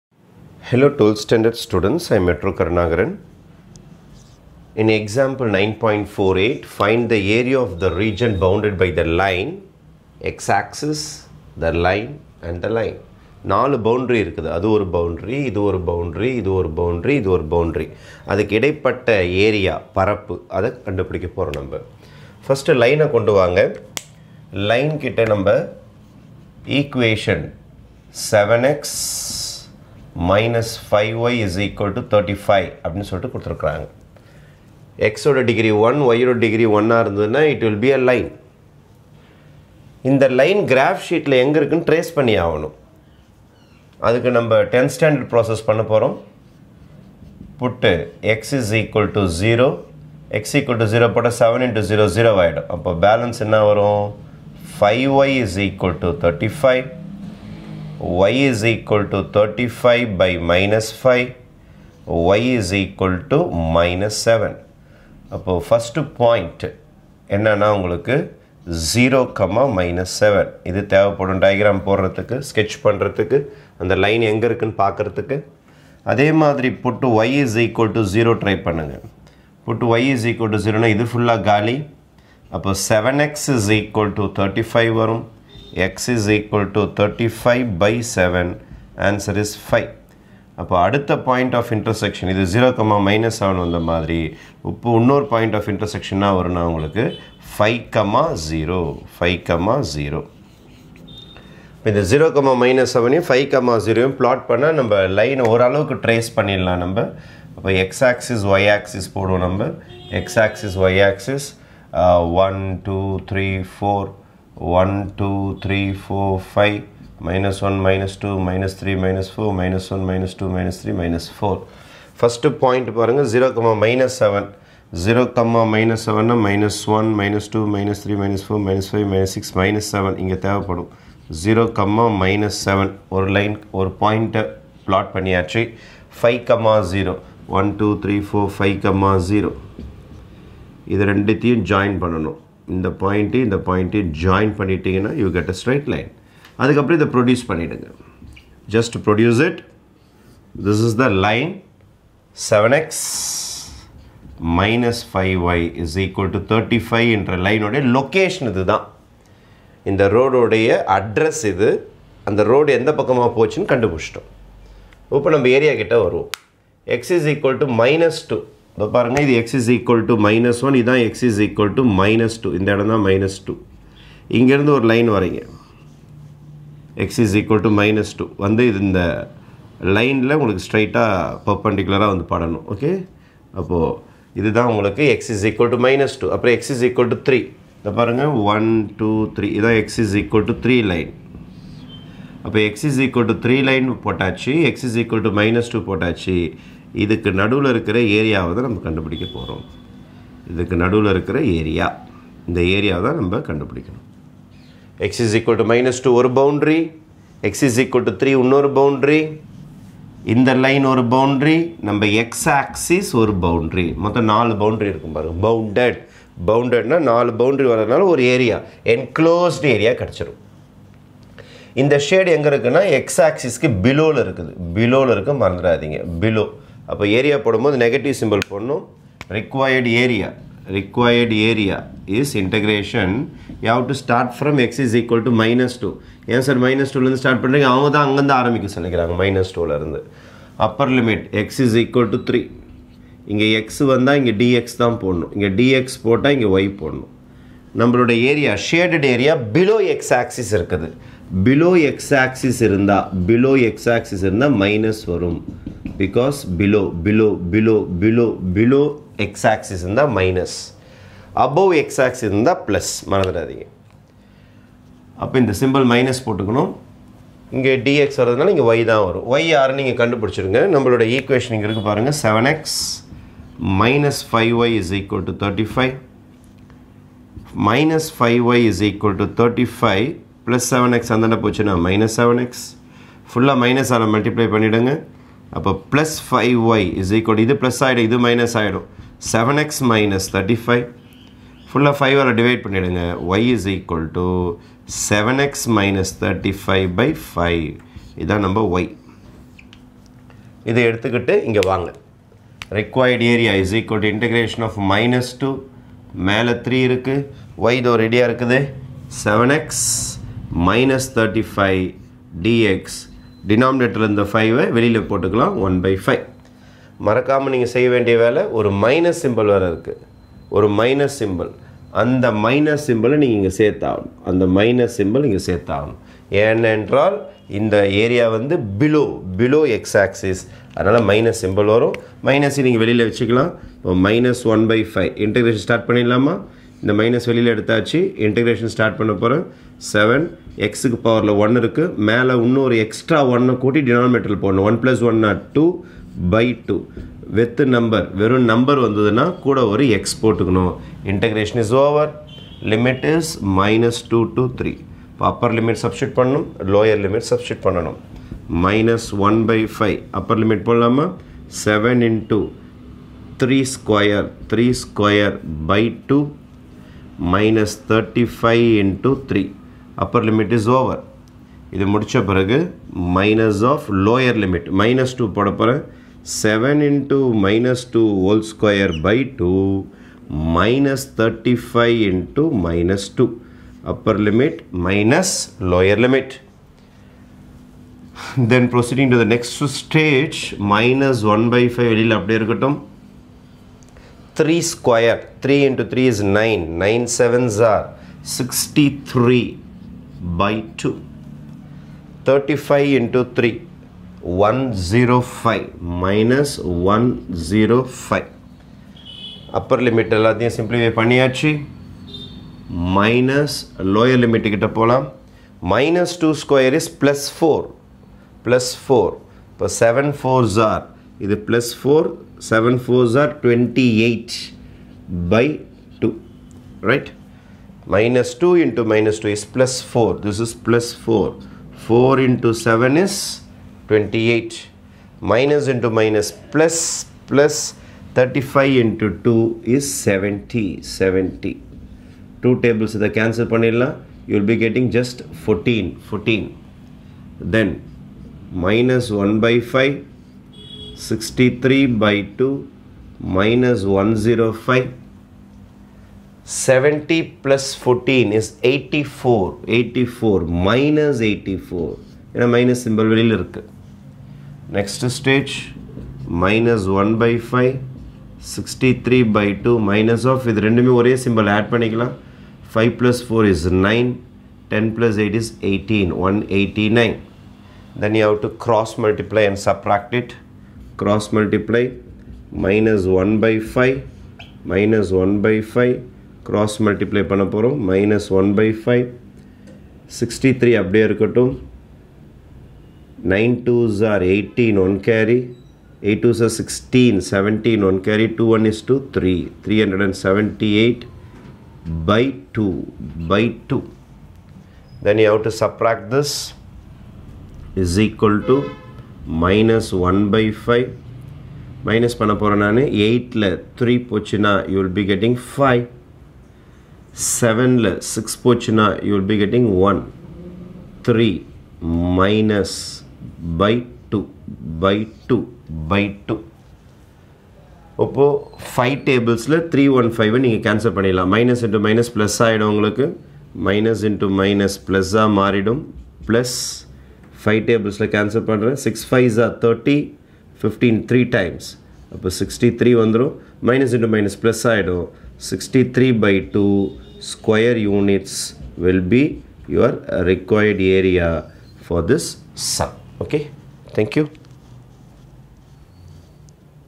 12th Std Maths Example 9.48 Find the area of the region bounded by line 7x-5y=35, x axis lines x=-2
12th Std MATHEMATICS

CHAPTER 9 Application of Integration
Examples